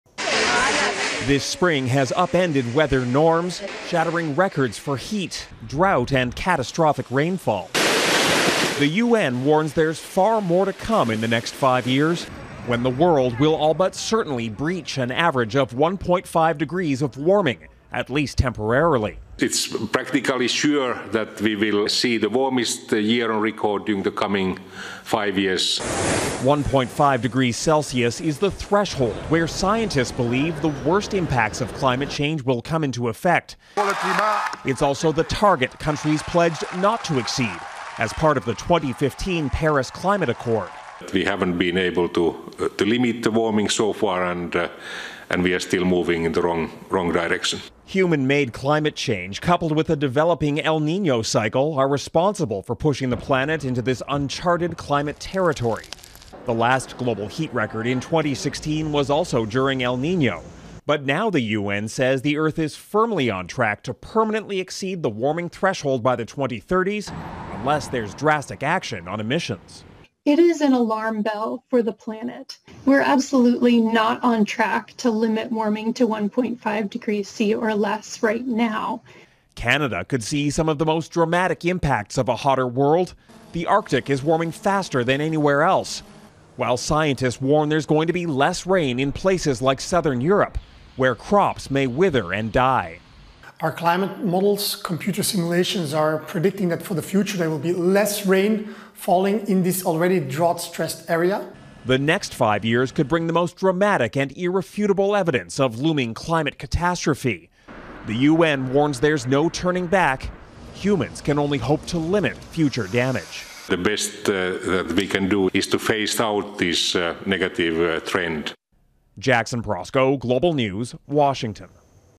Global warming "likely" to breach 1.5°C threshold within 5 years: UN
There's a 66 per cent chance Earth will break the crucial 1.5°C increase in global temperatures within five years, according to the World Meteorological Organization of the United Nations.

The 2015 Paris climate agreement set 1.5°C as an international threshold. While that may not seem like a lot, scientists say such a spike will prompt the worst impact of climate change.

Jackson Proskow explains the likely consequences.